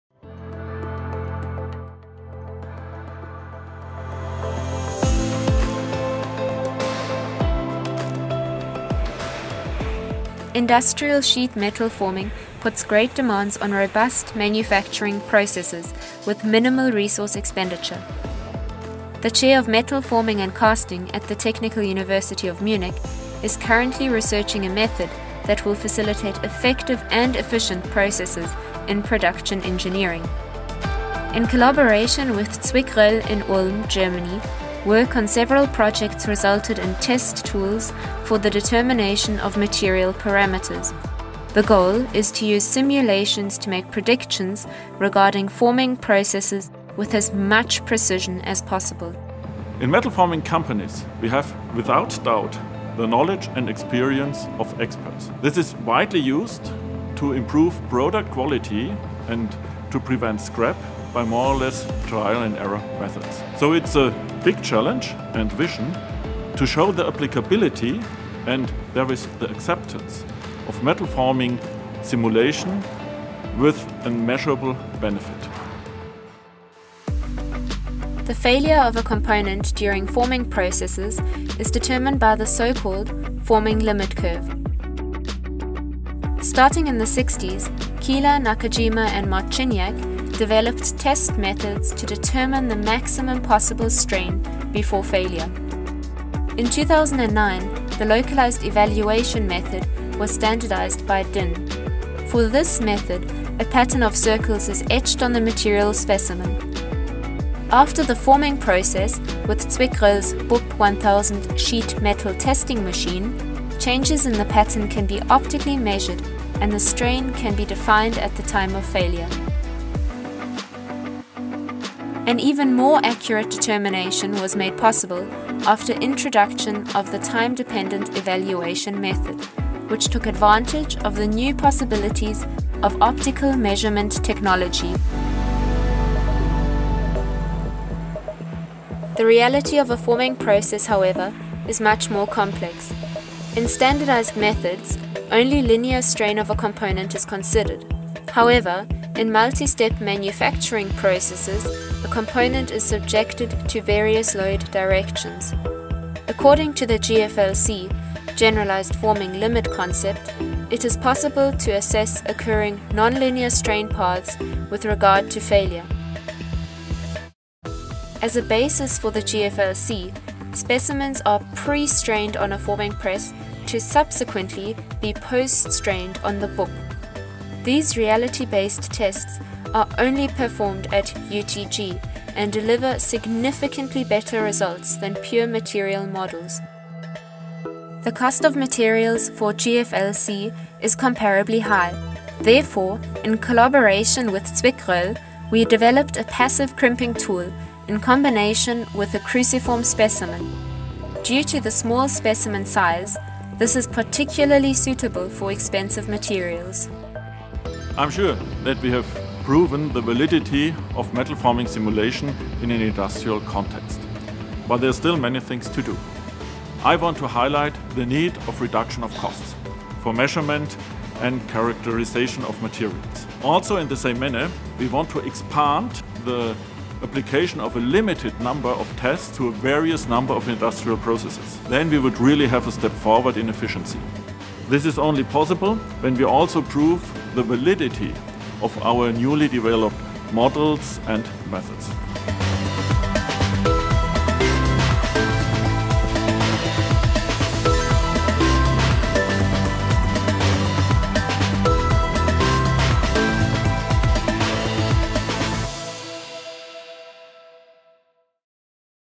Sheet metal testing research project - TU Munich (utg)
The Chair of Metal Forming and Casting (utg) at the Technical University of Munich, Germany (TUM) researches on sheet metal forming in order to further improve industrial sheet metal forming processes. For material characterization they use a ZwickRoell sheet metal testing machine BUP1000 to determine forming limit curves.

Copyright: utg / TUM